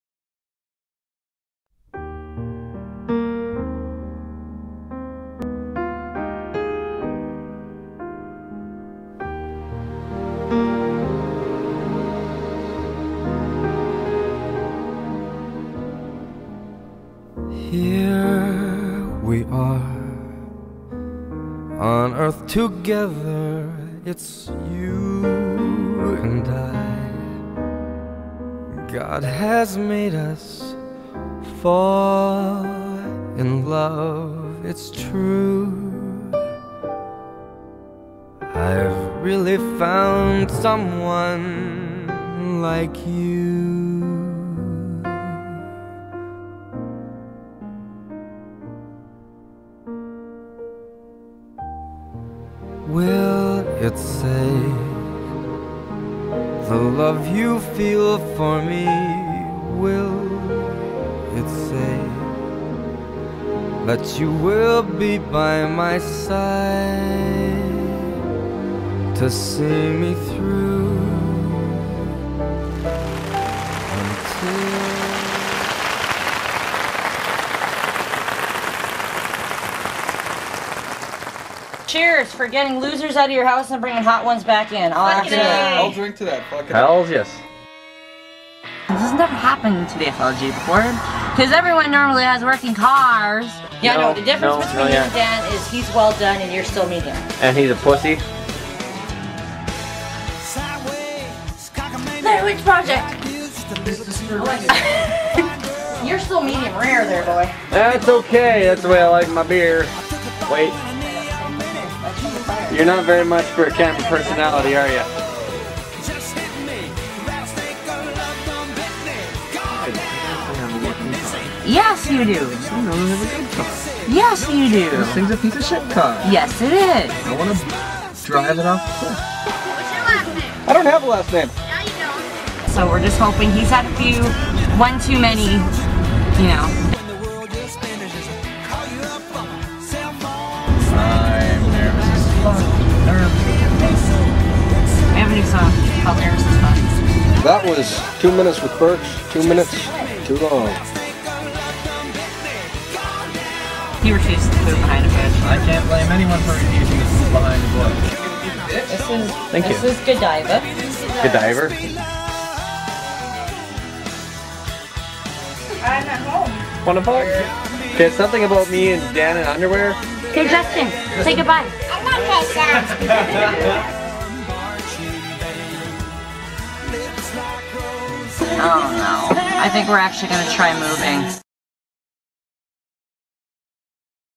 FLG Shred-your-Nuts Adventure Trailer
After a long bake in the hot, hot sun the day before, Froot Loops and Shreddies dared thjemselves to venture forth into the great outdoors once again.  Loops wimped out, but Shreds lasted the night, and it was a good thing he did as well.  Could it be that tonight's the night that the question is popped and him and Nuts become an Item?  Anyone who knows them knows DUH... and so to consummate this new found commitment... They go on a road trip to BC with Shreds mother.  To see Shreds SON of all things.   Oh my God... How many ways past screwed can you spell Doom?